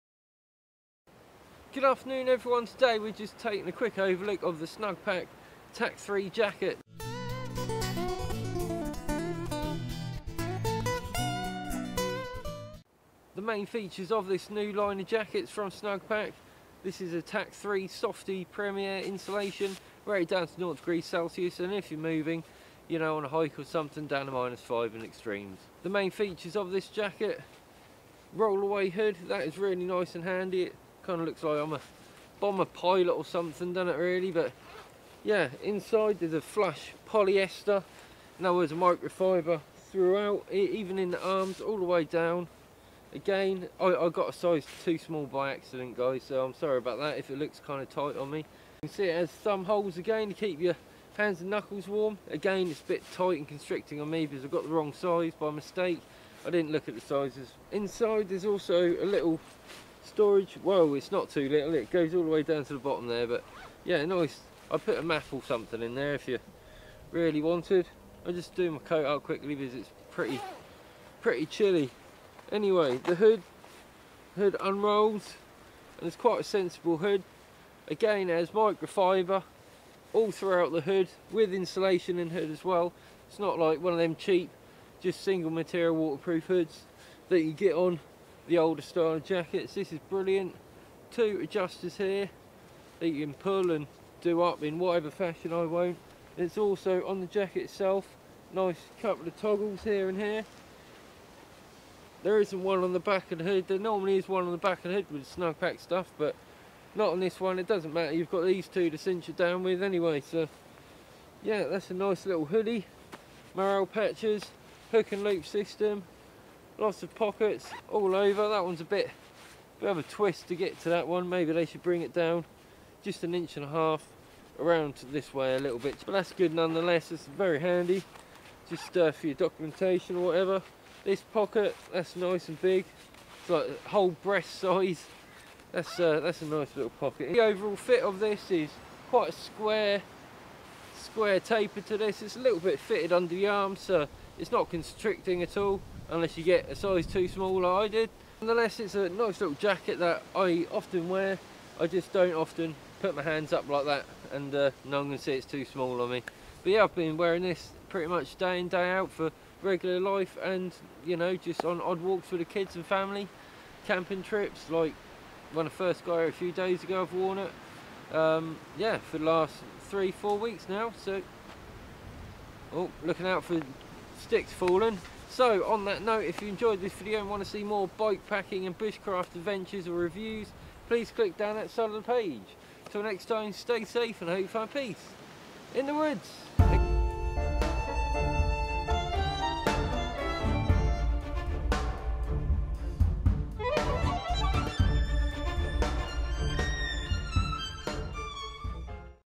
Snugpak TAC3 Jacket (review)
Today, I'm pleased to show the Lightweight Snugpak TAC3 Jacket which features a decent roll-away insulated hood, warm micro-fleece inner & Paratex materials throughout, with Softie premier insulation at the core, helping to keep you warm, wind-free & well ventilated, wicking away any moisture build-up. All in all, a great versatile lightweight jacket.